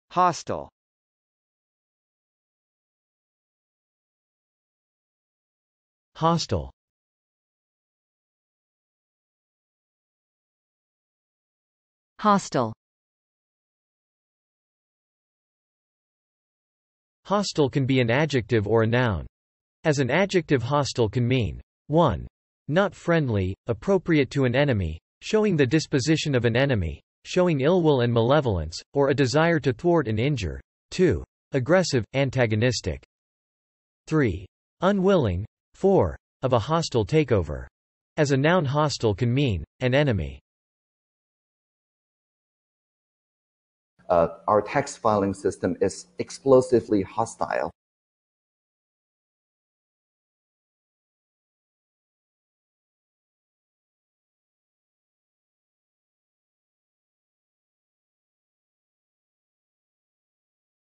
HOSTILE - Meaning and Pronunciation
How to pronounce hostile?

This video provides examples of American English pronunciations of hostile by male and female speakers.
In addition, it explains the meaning of hostile through a dictionary definition and several visual examples.

IPA Transcription of hostile is /hˈɑstəl/.

Definition of hostile:
hostile can be an adjective or a noun

As an adjective hostile can mean:
1. Not friendly, appropriate to an enemy; showing the disposition of an enemy; showing ill will and malevolence, or a desire to thwart and injure.
2. Aggressive, antagonistic.
3. Unwilling.
4. Of a hostile takeover.

As a noun hostile can mean:
An enemy.

If you'd like to test your pronunciation of hostile, check out Accent Hero's real-time pronunciation feedback tool: